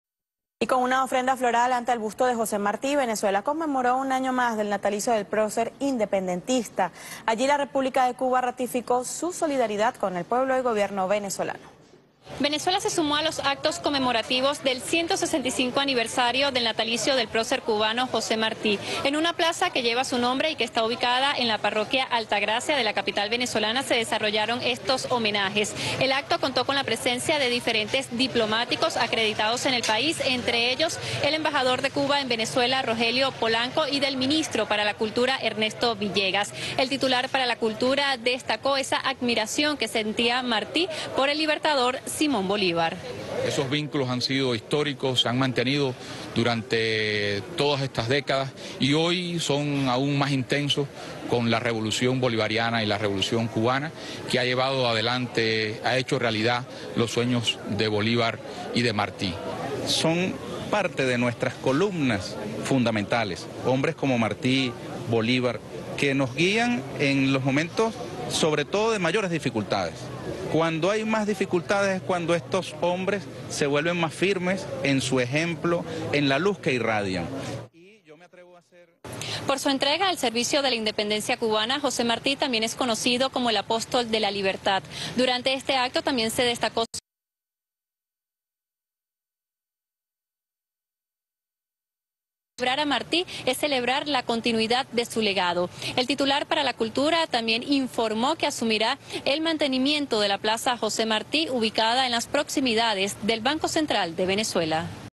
MInistro Villegas en acto homenaje a José Martí por su natalicio
Conmemoraron un año más del natalicio del prócer cubano José Martí, en la plaza que lleva su nombre en la parroquia Altagracia de Caracas.